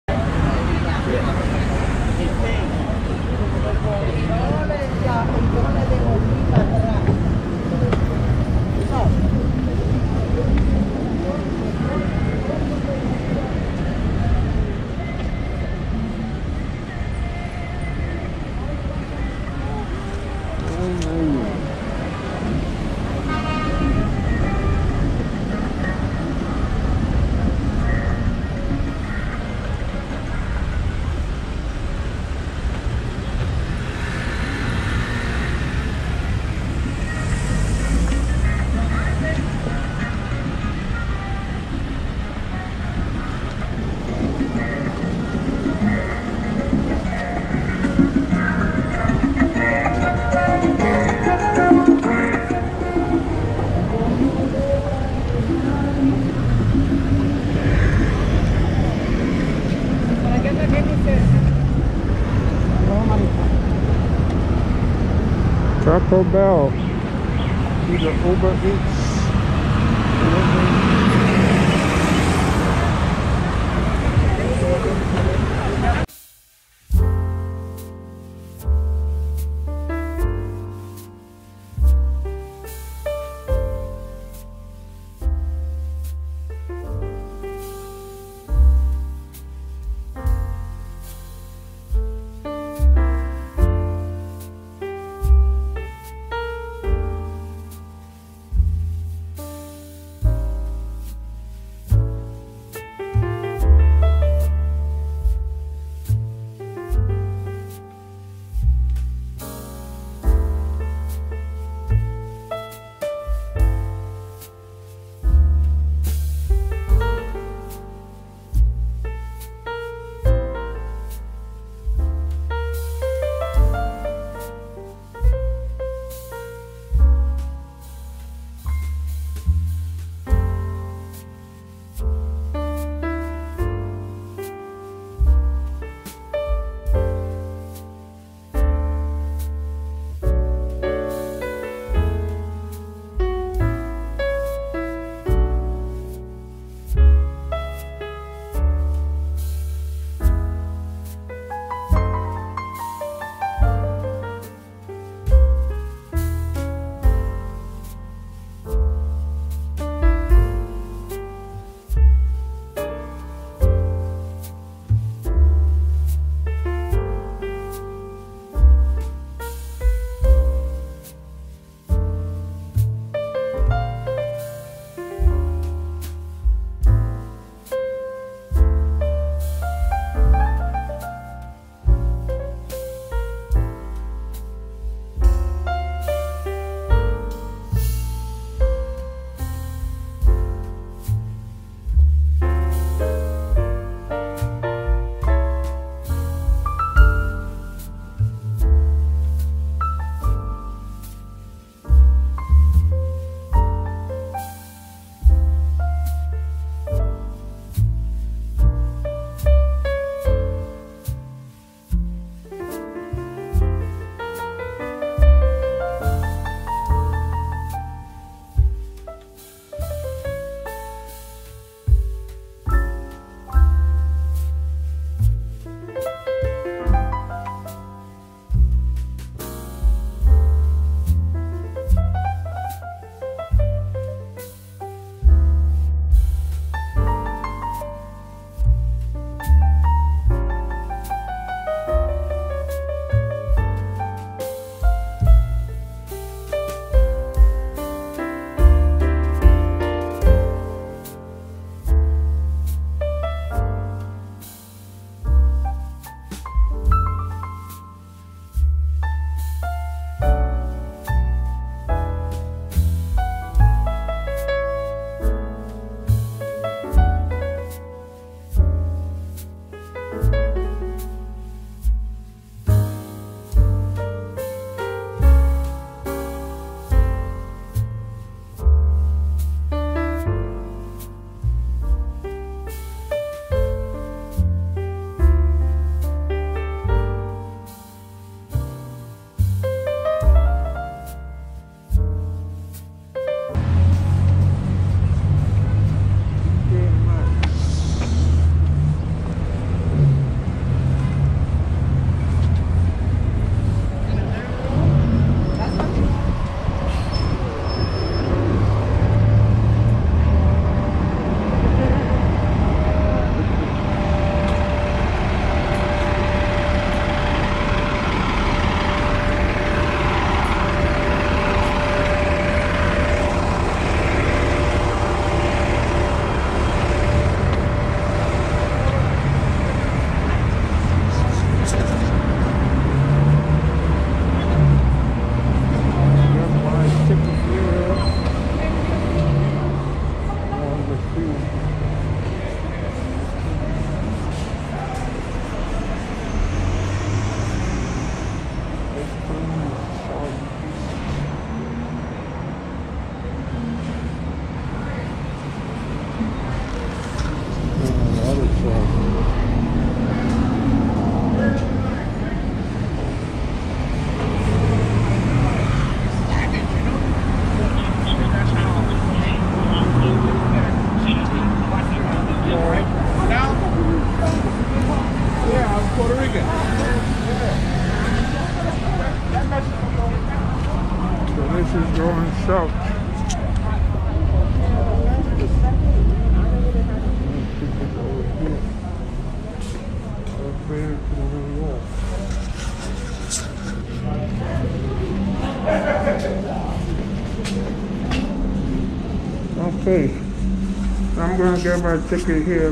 Checking Out Fordham Shopping District Part 3 of 3
Continue touring the shopping district on Fordham Road.

by Colbie Caillat